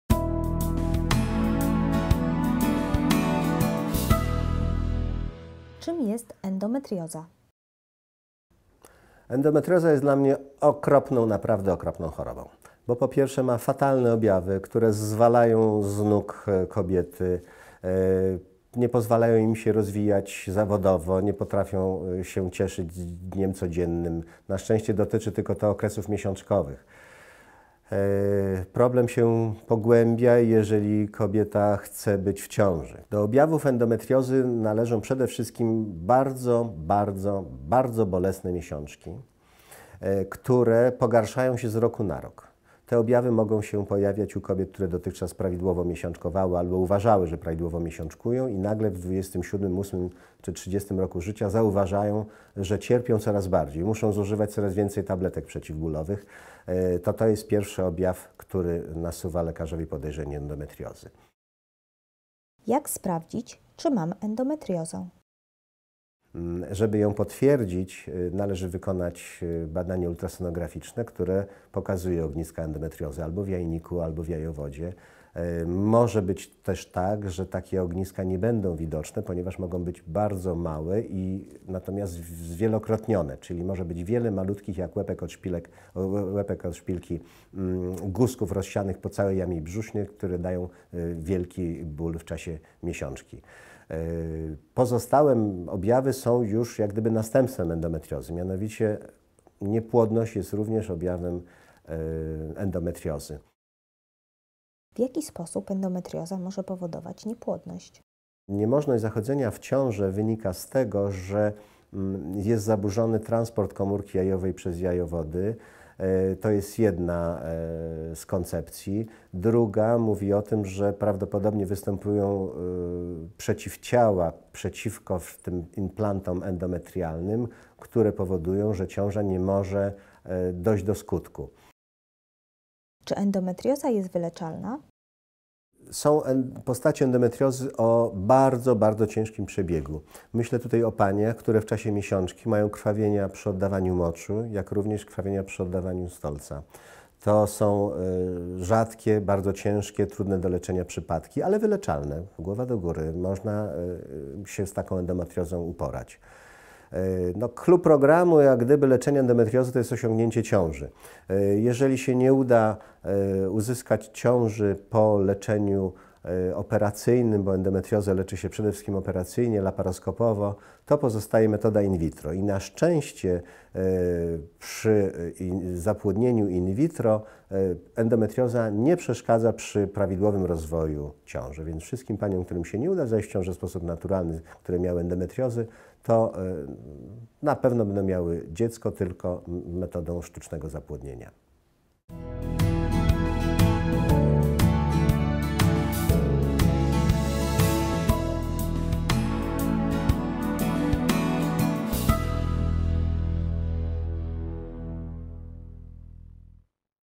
Czym jest endometrioza? | BabyBoom
Masz kłopot z zajściem w ciążę? Czy wiesz, że powodem może być endometrioza?
Czym jest, dlaczego jest groźna i jak się ją leczy?
Kolejny odcinek, w którym na pytania z odpowiada dr n. med. Tomasz Zając